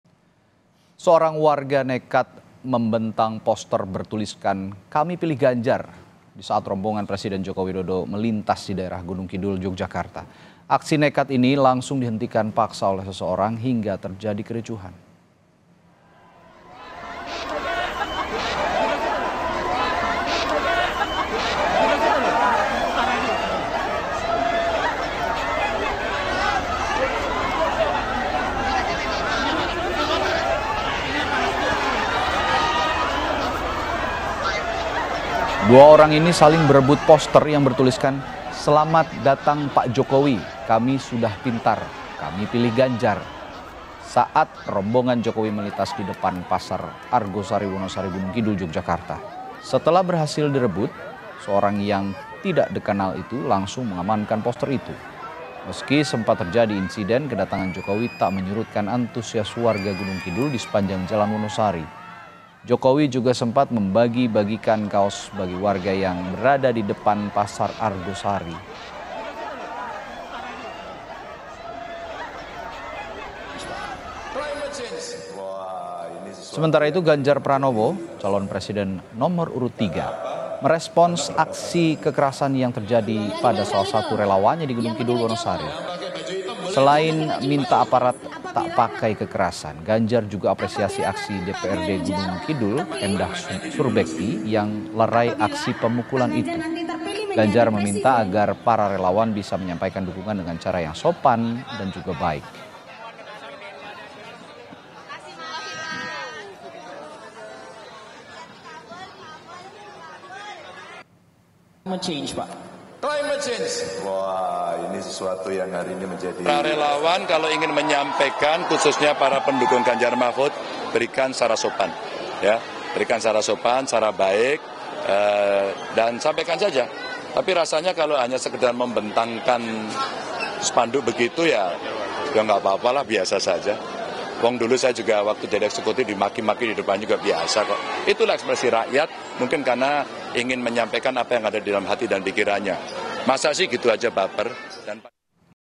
Aksi Warga Bentangkan Poster Pilih Ganjar saat Jokowi Lewat, Ini Kata Ganjar
KOMPAS.TV - Aksi Warga Bentangkan Poster Pilih Ganjar saat Jokowi Lewat, Ini Kata Ganjar

Kompas TV
Independen | Terpercaya